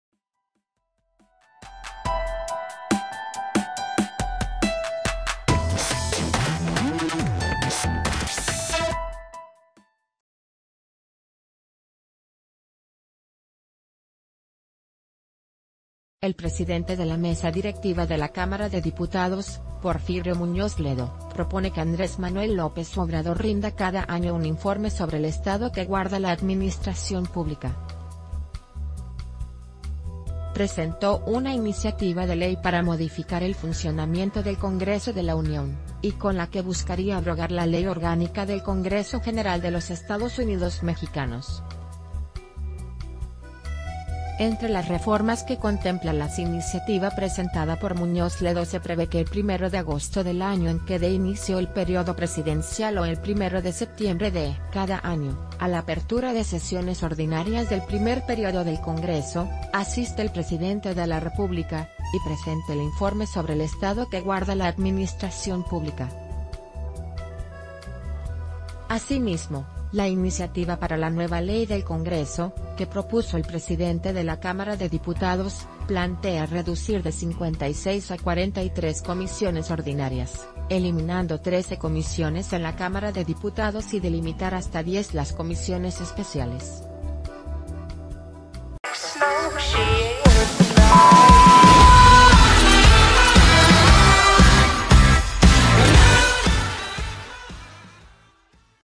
Muñoz Ledo propone que AMLO rinda informe cada año
El presidente de la Mesa Directiva de la Cámara de Diputados, Porfirio Muñoz Ledo, propone que Andrés Manuel López Obrador rinda cada año un informe sobre el estado que guarda la administración pública. Presentó una iniciativa de Ley para modificar el funcionamiento del Congreso de la Unión y con la...